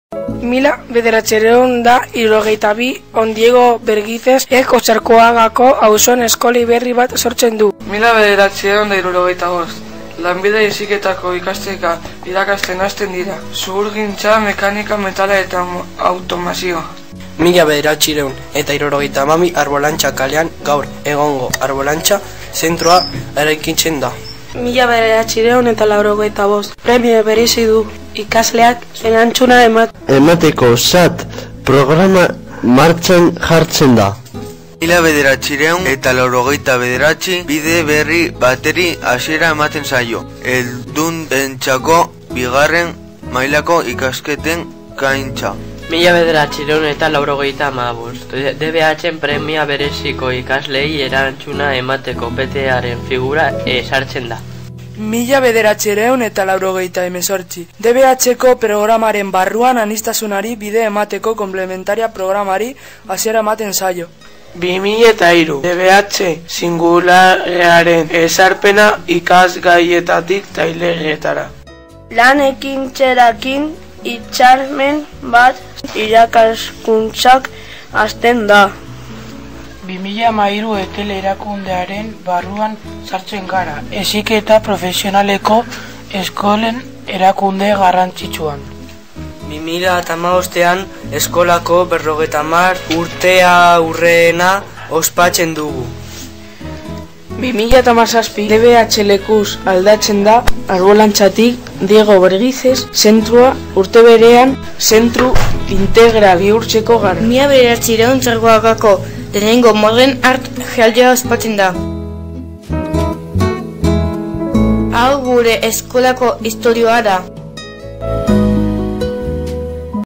Jazotakoak jaso Otxarkoaga (Elizbarrutiko ikastetxeentzako bideoklipen lehiaketa)
Video para el concurso de los centros diocesanos, contado la historia de la Escuela , realizado por los alumnos de 1B de la ESO, con la colaboracion de Tular Irratia